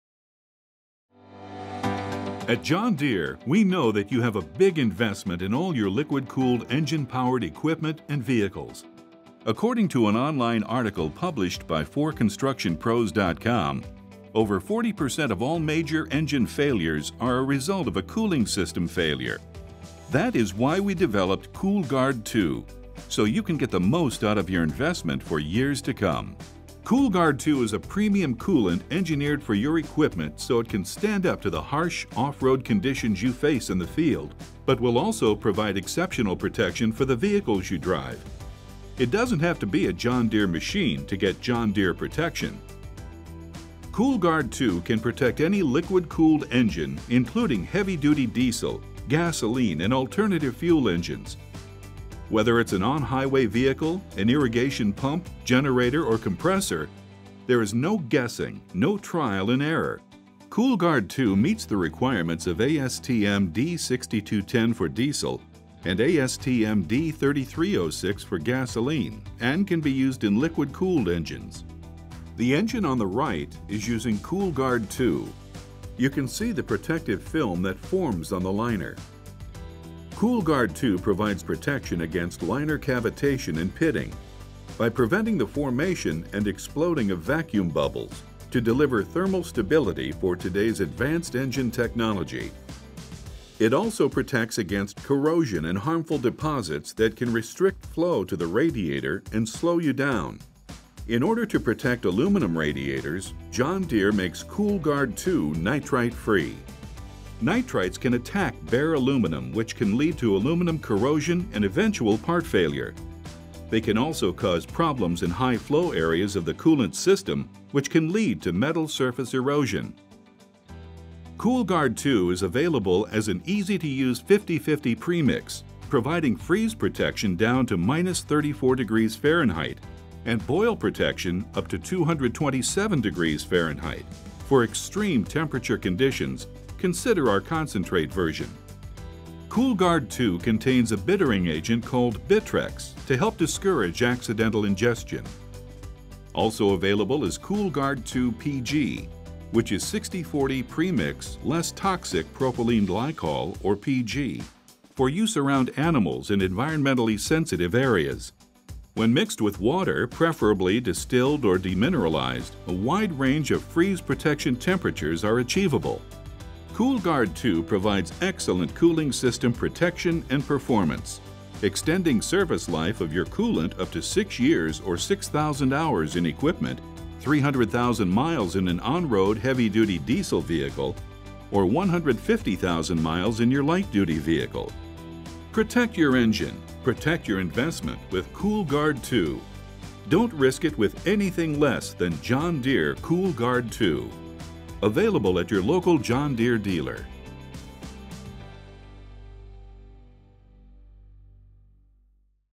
Cool-Gard™ II Educational Video - English
Cool-Gard II provides excellent cooling system protection and performance, extending service life up to 6-years or 6,000 hours in equipment, 300,000 miles in an on-road, heavy-duty diesel vehicle or 150,000 miles in your light-duty vehicle.